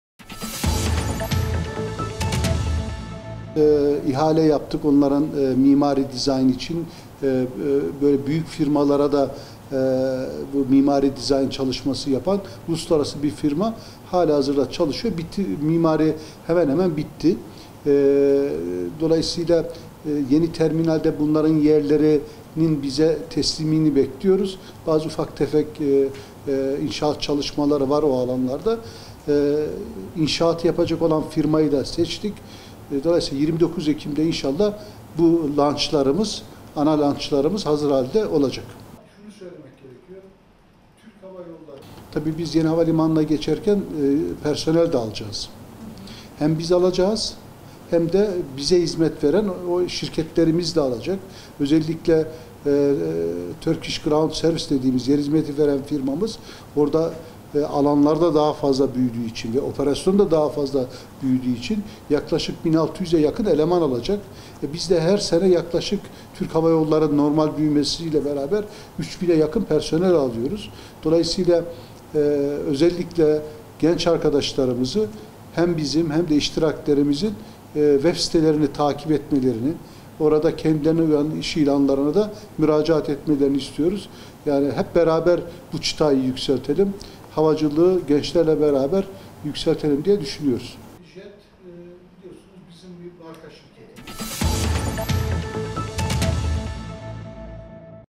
THY ve iştiraklerine 4 bin 600 personel alınacak
Türk Hava Yolları (THY) Genel Müdürü Bilal Ekşi, İstanbul Yeni Havalimanı'nda alanın büyümesi ve operasyonların artmasına bağlı olarak THY ve iştiraklerine 4 bin 600’a yakın personel alınacağını belirtti.